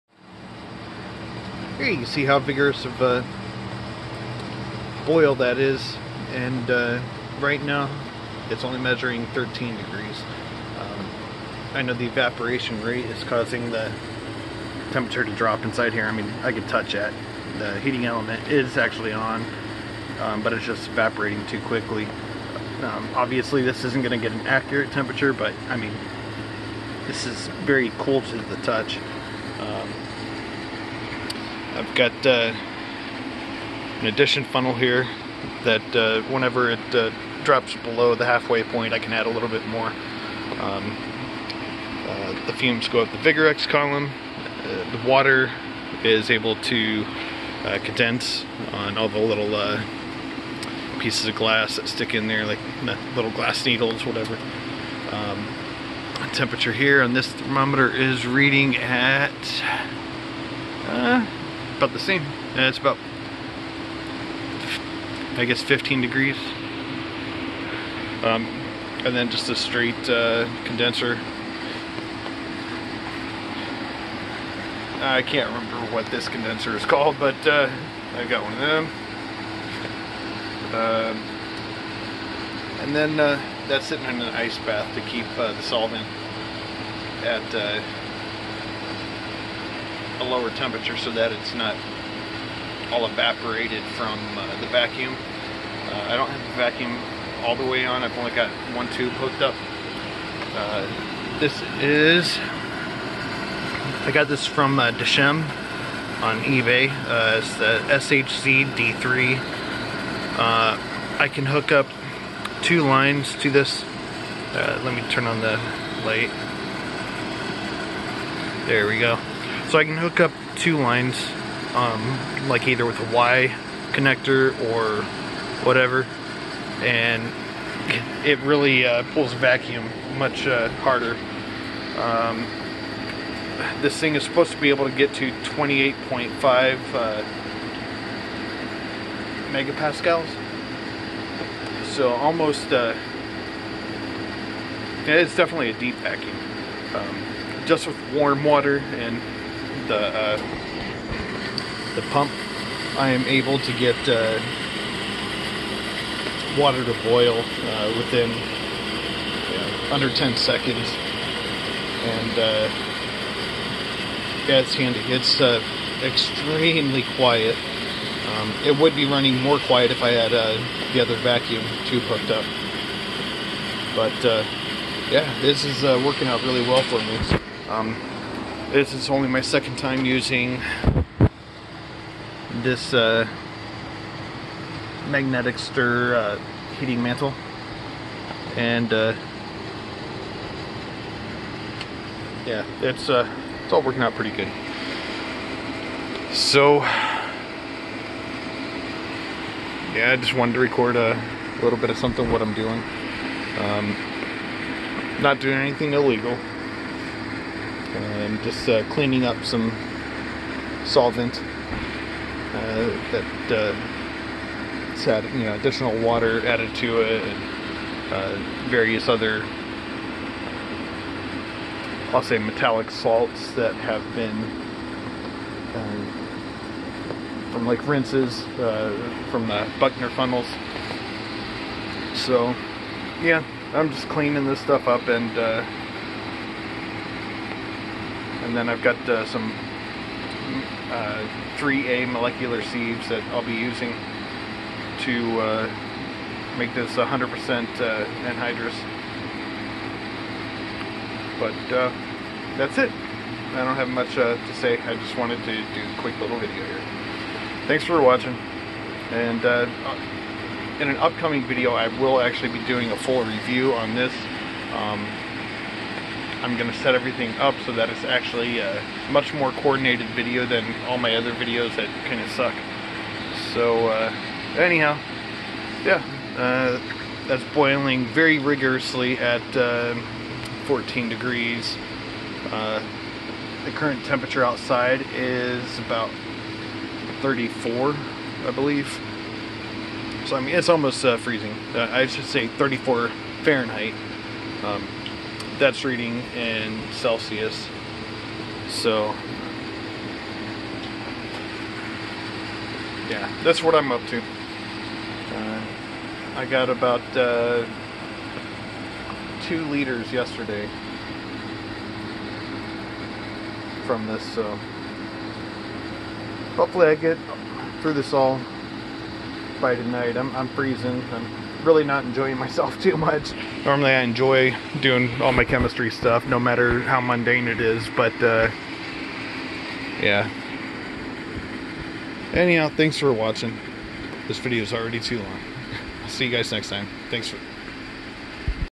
Home Chemistry: Organic Solvent Vacuum Distillation
Organic solvents (i.e. alcohols) can be expensive, however much of the used solvent can be cleaned or reclaimed by distillation.  The solvent I was distilling forms an azeotrope at around 96%, which is the original strength of the product.  I will by drying the solvent with 3A molecular sieves.

I'm using a water circulating vacuum pump to both cool the condensers and to lower the atmospheric pressure of the distillation apparatus.